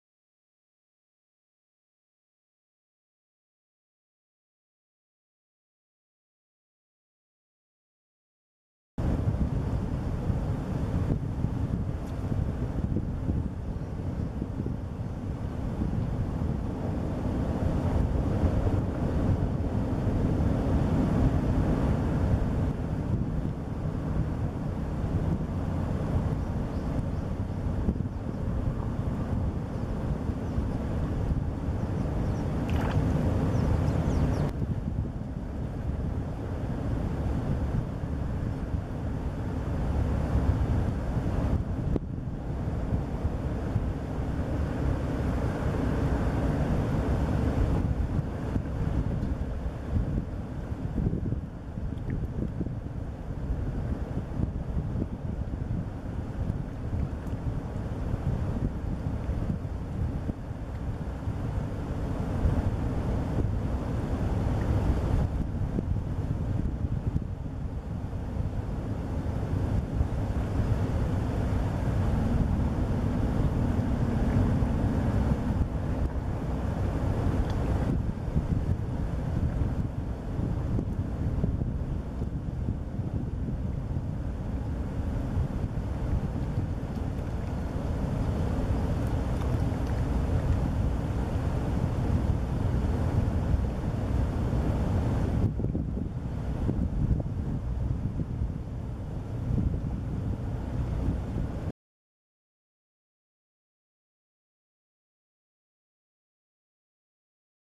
GARZA AZUL (Egretta caerulea) LITTLE BLUE HERON
GARZA AZUL (Egretta caerulea)

Una Garza comun en Puerto Rico y a traves de las Antillas. Es una de las garzas más acuaticas que tenemos, esta prefiere aguas poco profundas, campos inundados y piscinas de agua fresca con vegetación para cazar. Su dieta por lo general consiste de invertebrados y peces pequeños. En su etapa juvenil su plumaje es color blanco, mientras esta va desarrollandose a adulta el plumaje se torna un gris azulado.

LITTLE BLUE HERON (Egretta caerulea)

A common resident heron species in Puerto Rico and throughout the West Indies. It is one of our most aquatic herons, preferring fresh water pools, shallow waters, flooded fields and pools with vegetation, to hunt for small invertebrates and fish. When young its plumage is white morphing to a bluish gray when fully matured.